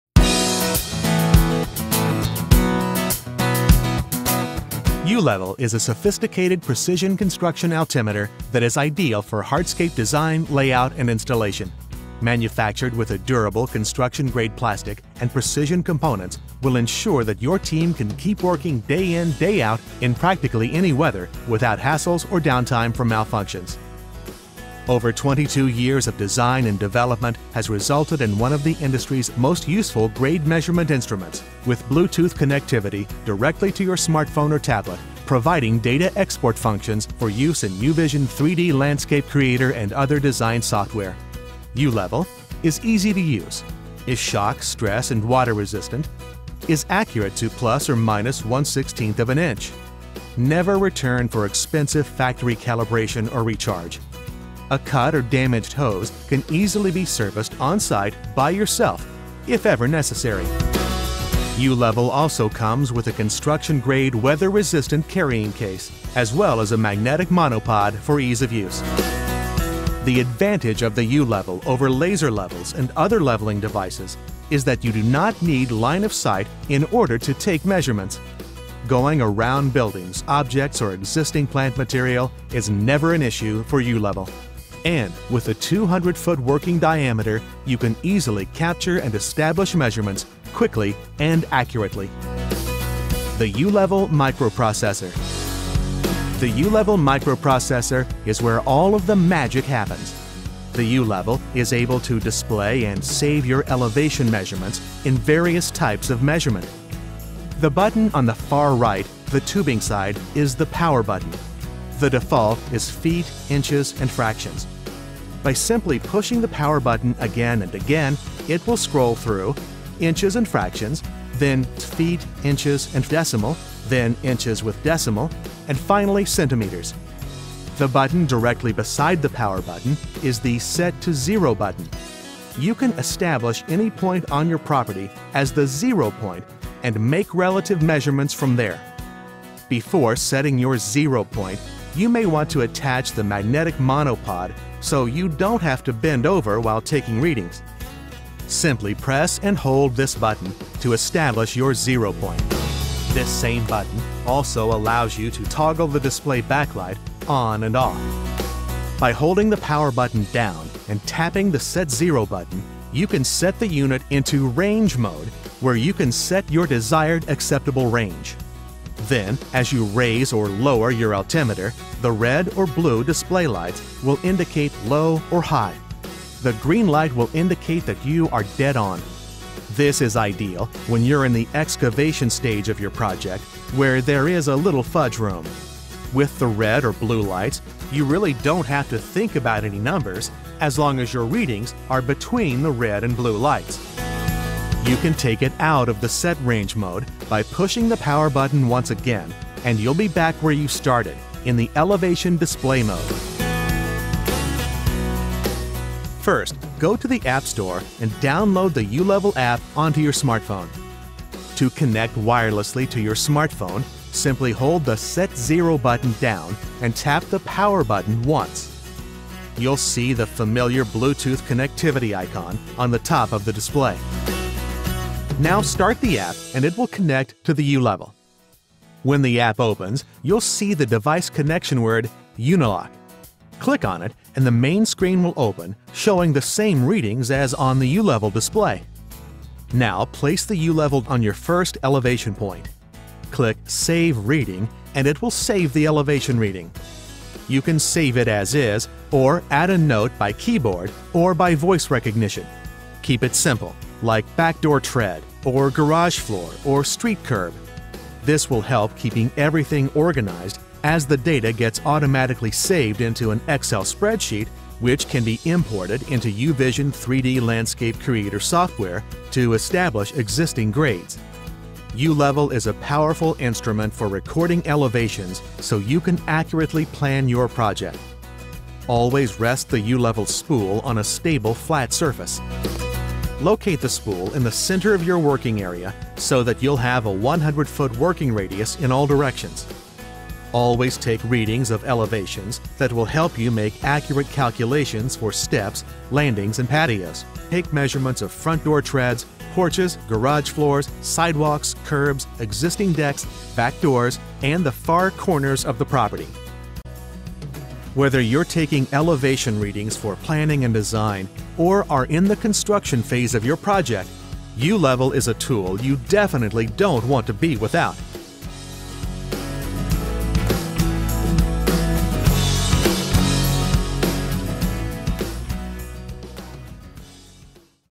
U-Level - The Precision Digital Hardscape Level
U-Level is a precision elevation measurement tool that is ideal for hardscape design, layout and installation. It has become  one of the industry’s most useful grade measurement instruments, with Bluetooth connectivity directly to your smartphone or tablet, providing data export functions for use in U-Vision 3D Landscape Creator and other design software.

𝗪𝐚𝐧𝐭 𝐭𝐨 𝐥𝐞𝐚𝐫𝐧 𝐦𝐨𝐫𝐞? 𝐕𝐢𝐬𝐢𝐭 𝐭𝐡𝐞 𝐅𝐀𝐐𝐬 𝐬𝐞𝐜𝐭𝐢𝐨𝐧

𝐅𝐨𝐥𝐥𝐨𝐰 𝐮𝐬 𝐟𝐨𝐫 𝐦𝐨𝐫𝐞 𝐠𝐫𝐞𝐚𝐭 𝐜𝐨𝐧𝐭𝐞𝐧𝐭 𝐚𝐧𝐝 𝐢𝐧𝐬𝐩𝐢𝐫𝐚𝐭𝐢𝐨𝐧!